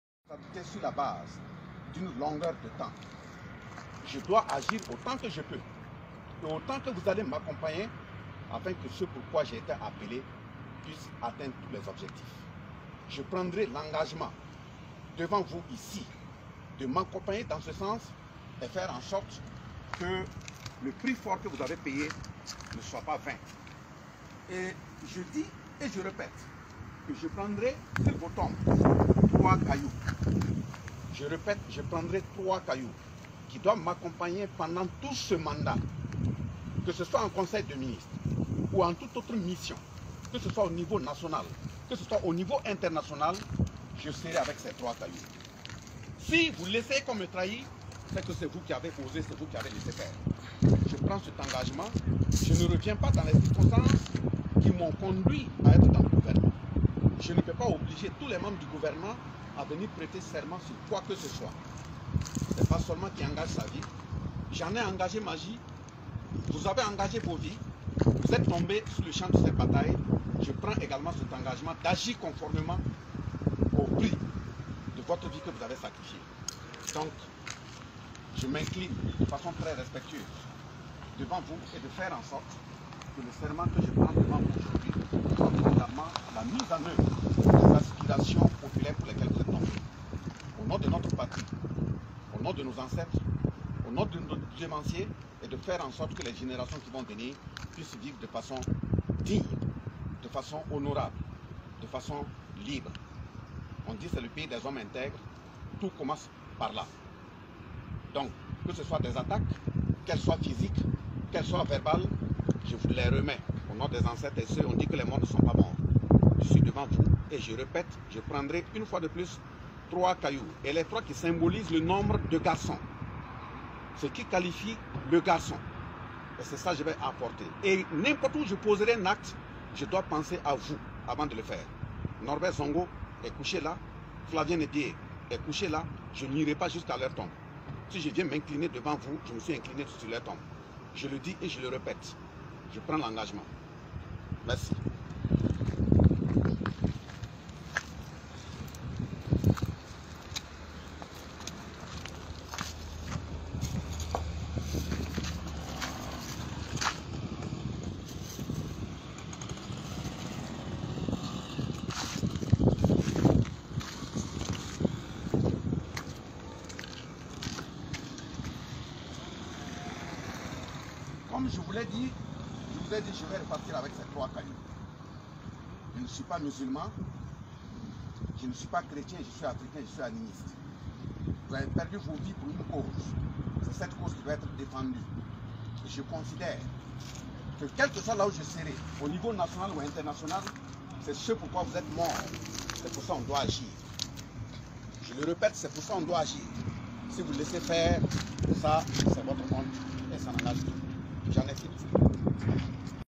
Voici le nouveau Président du Burkina Faso jurer devant la tombe de Sankara et prend trois cailloux
Le nouveau Président du Faso jure devant les tombes de Sankara et autres nationalistes de l'aider à ne pas faillir, de ce fait, il y prend trois cailloux qui vont l'accompagner partout.